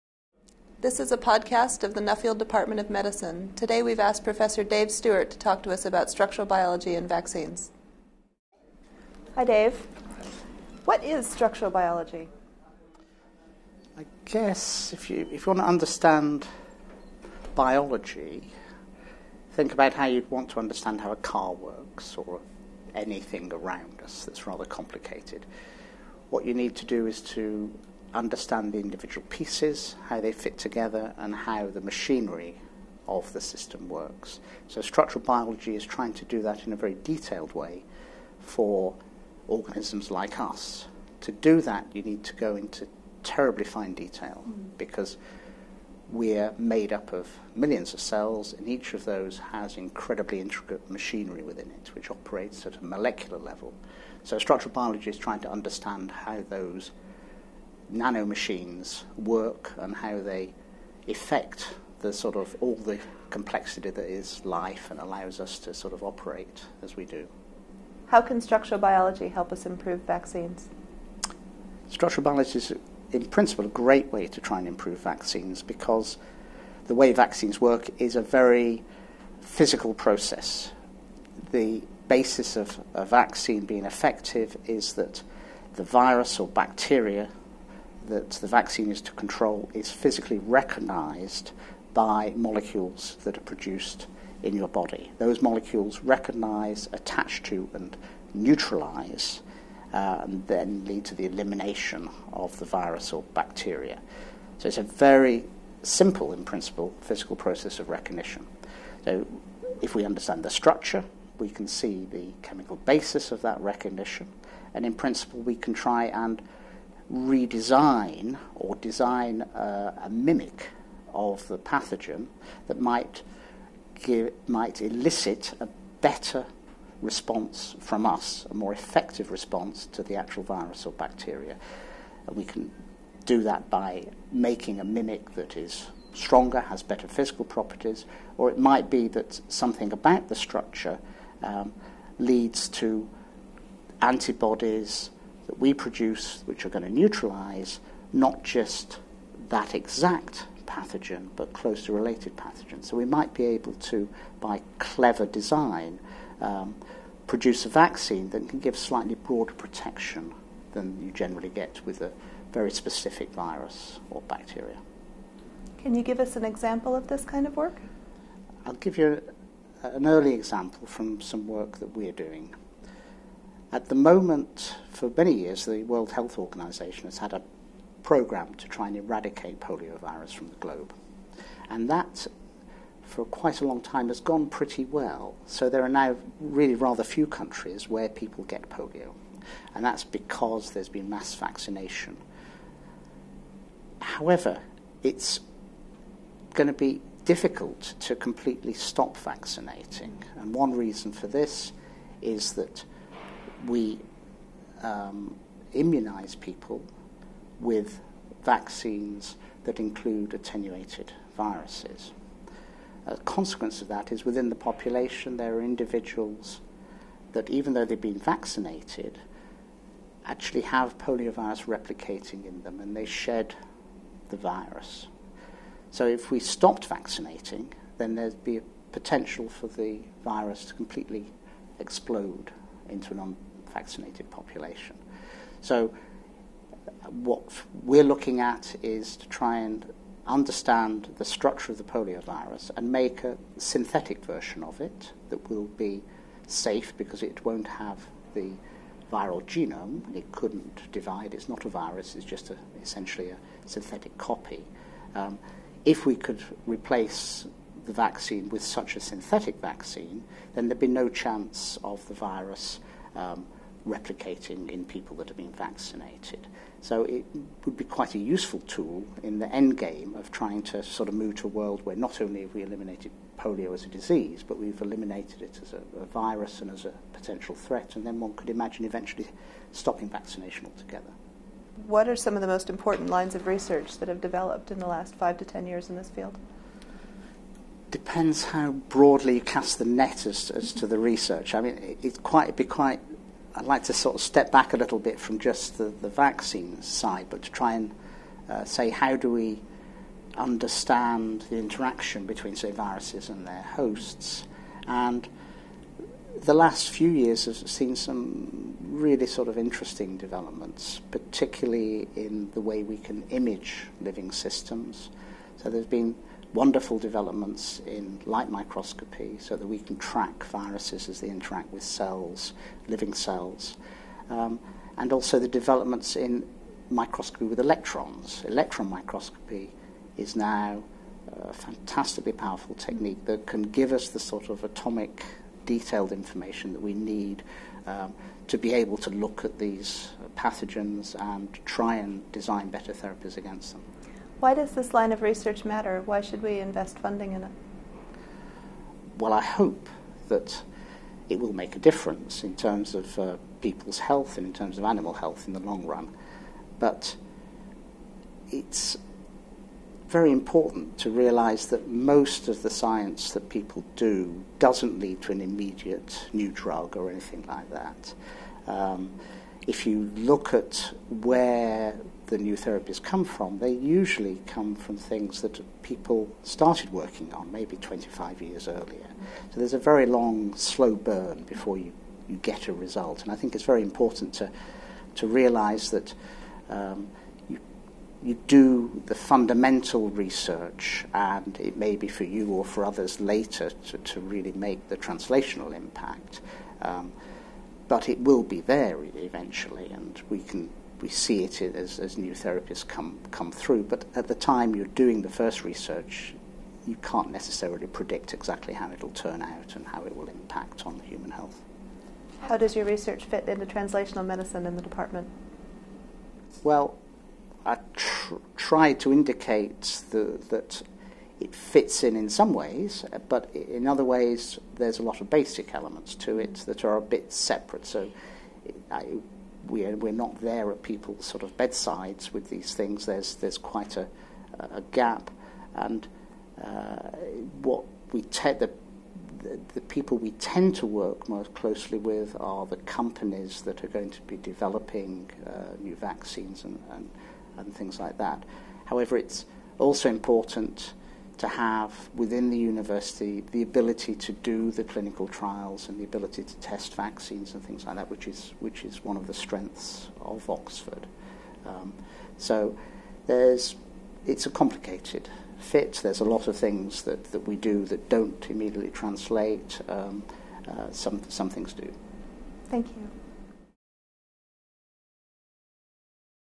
David Stuart: Structural biology & vaccines
Professor David Stuart studies the structure of viruses at the molecular level. His work is particularly interested in virus-receptor interaction and the basic puzzles of virus assembly and he uses structural biology to answer these questions.